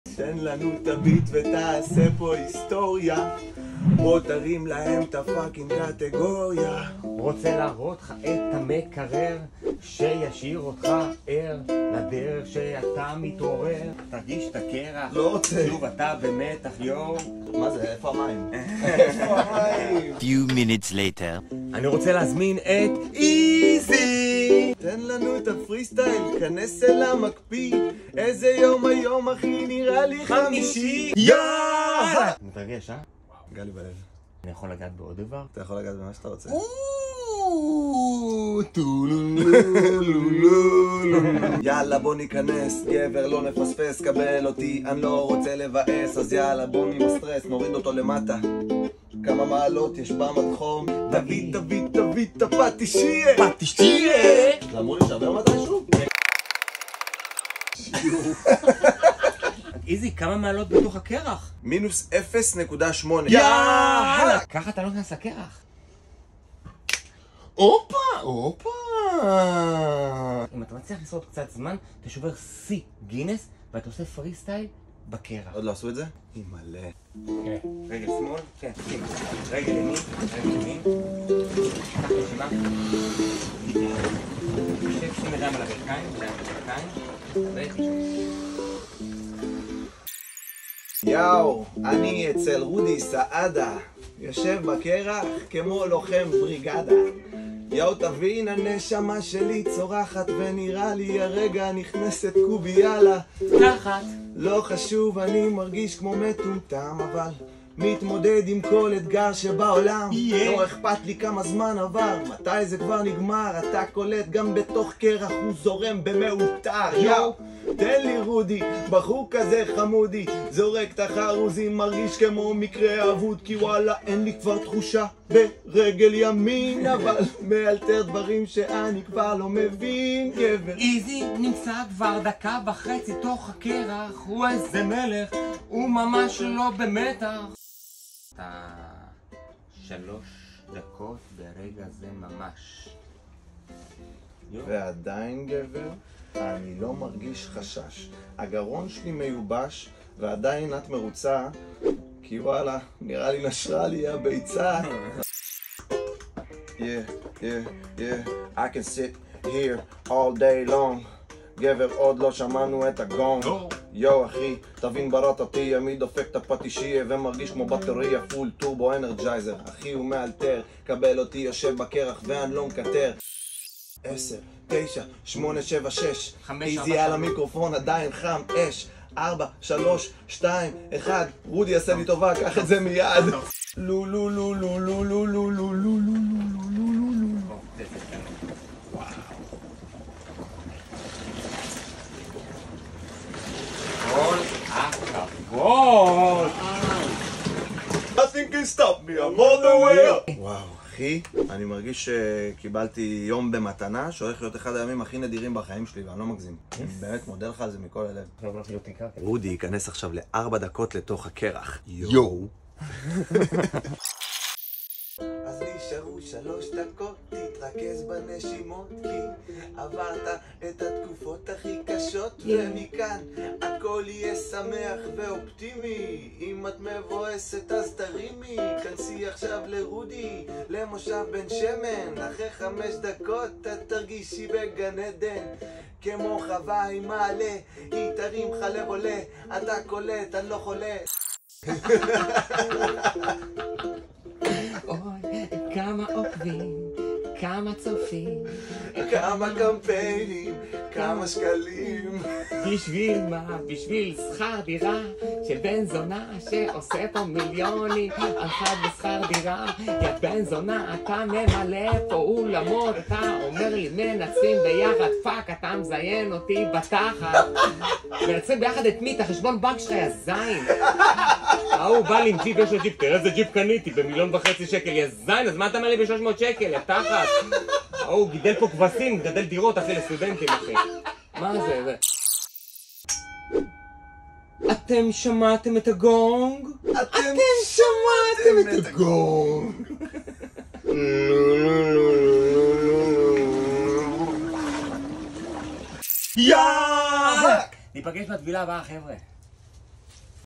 אתגר הקרח עונה 2 - טבילה 47 מתוך 100
הסטנדאפיסט והשחקן רודי סעדה, ממשיך לעונה 2 של אתגר הקרח - עונת החורף -  מסע של 100 ימים בתוך מקפיא קרח.
טבילה 47 מתוך 100.
מיום ליום יעלו פה הטבילות